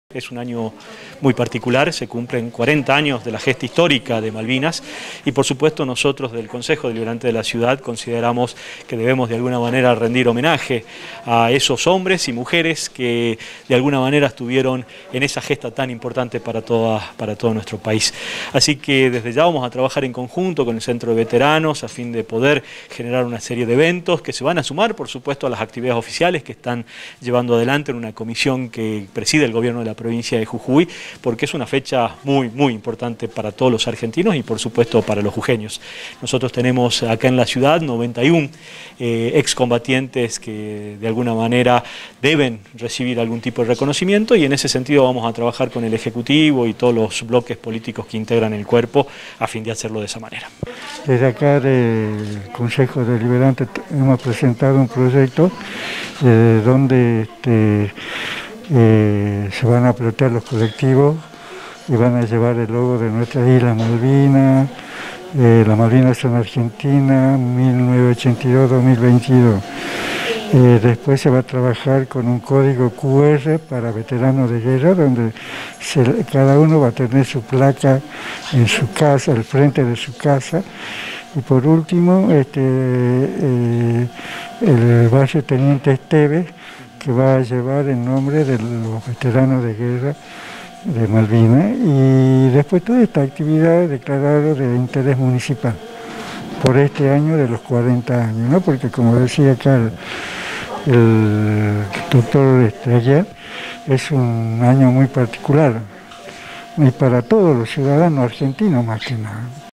Reunión por agenda con Veteranos de Malvinas FINAL
Ediles capitalinos se reunieron el pasado martes con el presidente del Centro de Veteranos de Guerra de Malvinas en Jujuy, Paulino Guanuco, con el objetivo de abordar diferentes iniciativas que se están evaluando para acompañar el 40° Aniversario de la gesta en el Atlético Sur.  La jornada tendrá una variada gama de actividades, con el objetivo de honrar a los ex combatientes que residen en nuestra ciudad.

Al respecto, el presidente del Concejo Deliberante, Dr. Lisandro Aguiar, afirmó que “queremos agradecer la presencia del Centro de Veteranos de Malvinas en la persona de Don Paulino Guanuco, este es un año particular, se cumplen cuatro décadas de la gesta histórica de Malvinas y es por eso que desde el Concejo Deliberante consideramos que debemos de alguna manera rendir homenaje a esos hombres y mujeres que estuvieron en el atlántico sur”.